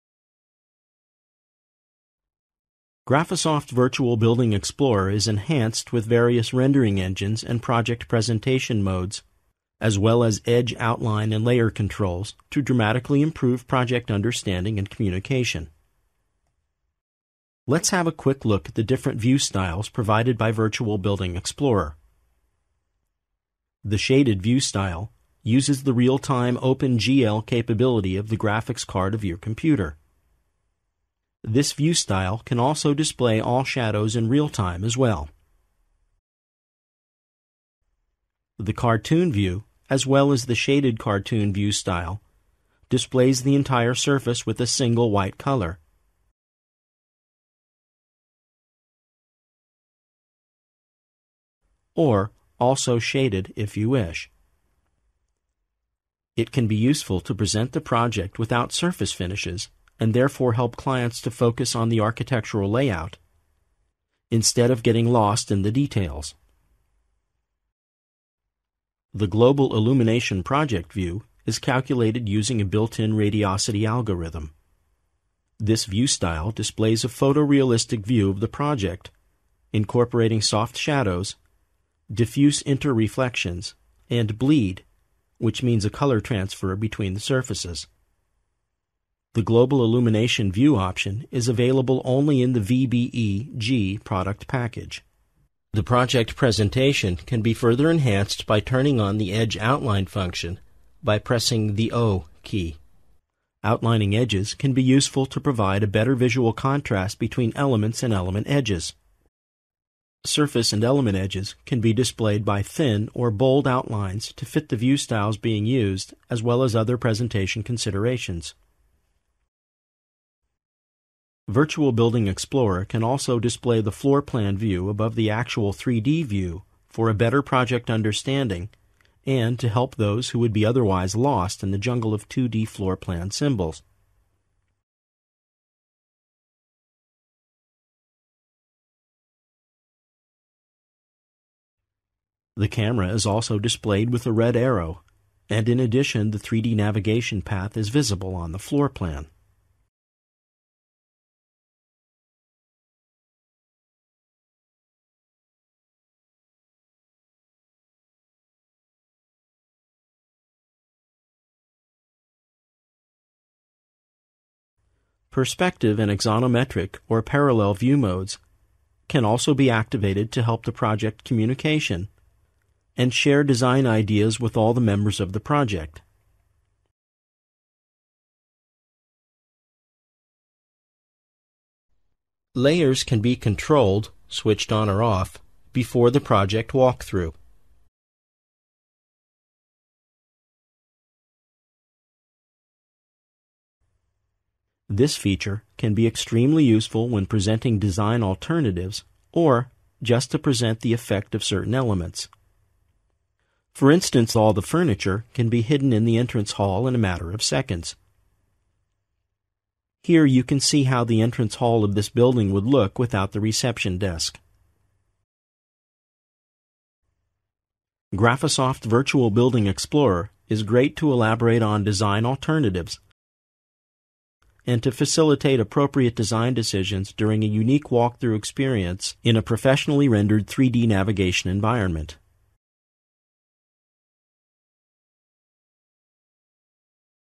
GRAPHISOFT Virtual Building Explorer for ArchiCAD 14 - Communicate
The GRAPHISOFT Virtual Building™ Explorer is an innovative, interactive, 3D presentation tool for architects, which sets new standards for design communication and presentation.

Virtual Building Explorer provides shaded, Open-GL or Global Illumination view styles combined with grayscale, hidden-line or edge-outlined presentation modes for the ultimate design understanding and communication.
The Sun altitude and azimuth; the camera height and viewing cone as well as the image brightness and transparency can be altered for professional project presentation and design communication.

Project credits:
NHS Office Complex,
paa studio, Pacific Palisades, CA